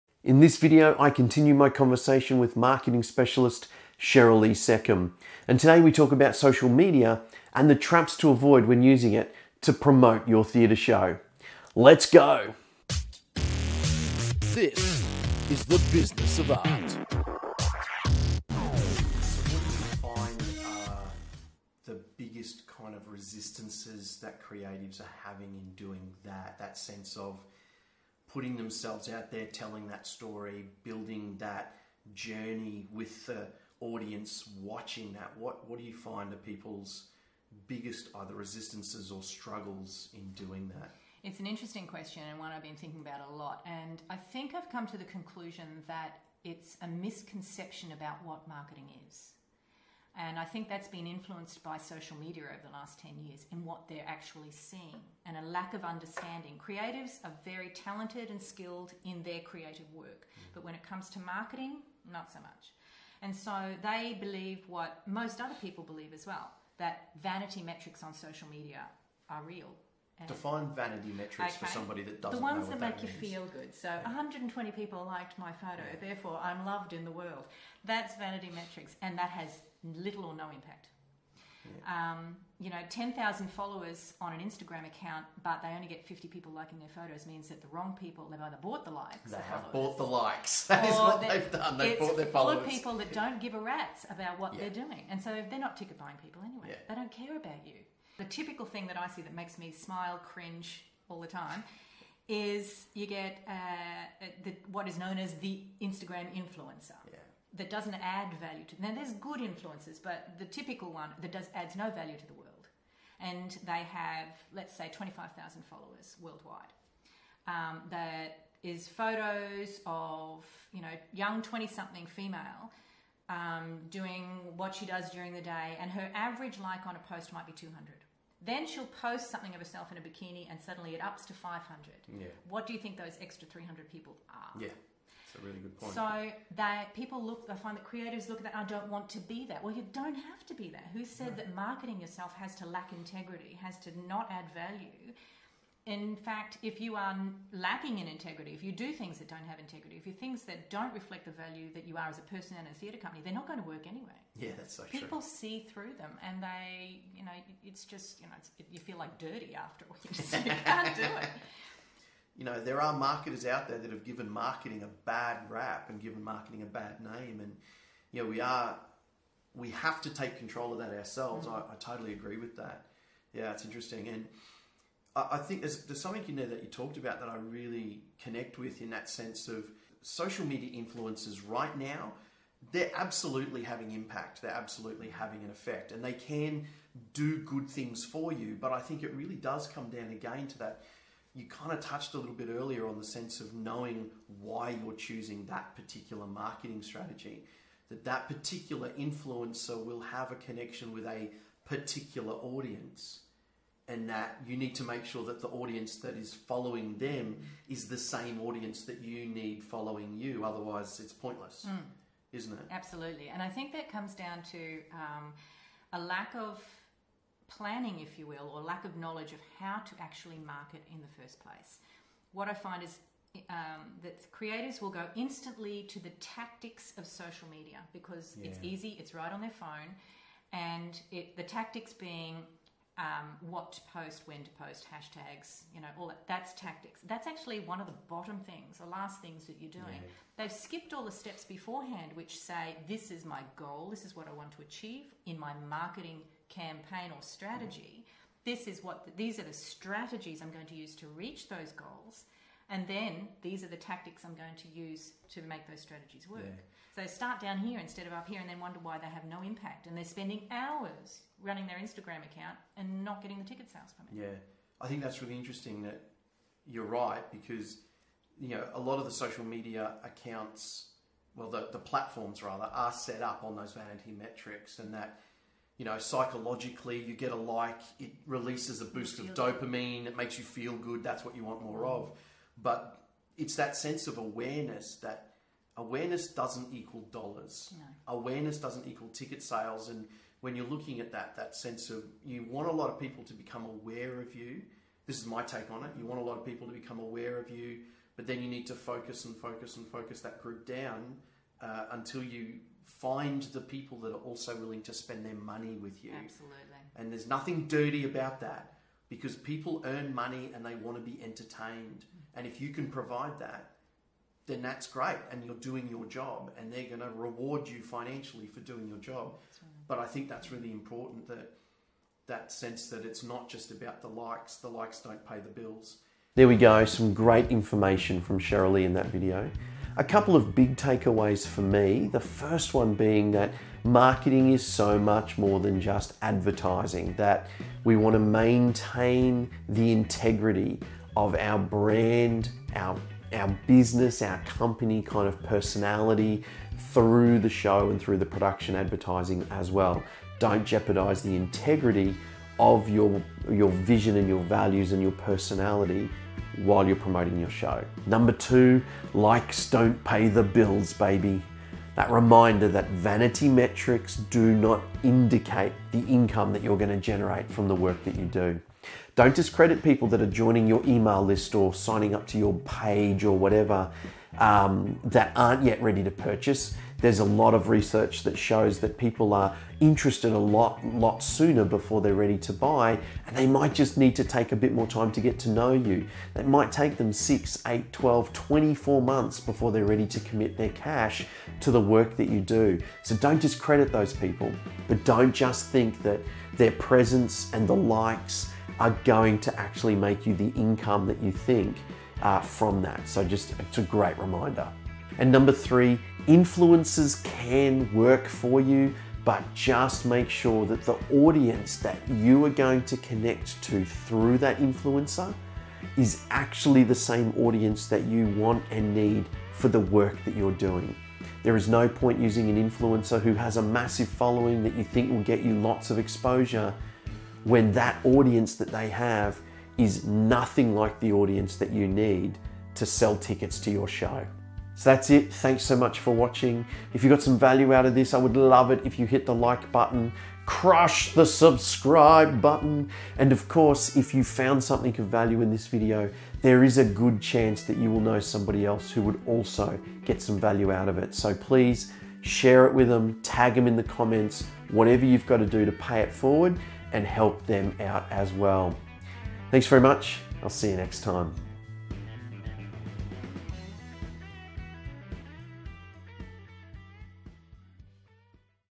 How NOT to use Social Media - Marketing Theatre Part 2
In part 2 of my interview with marketing specialist Sherryl-Lee Secomb we chat about some important things to consider when using social media to market your theatre company or production.

Not sure where to start learning about marketing?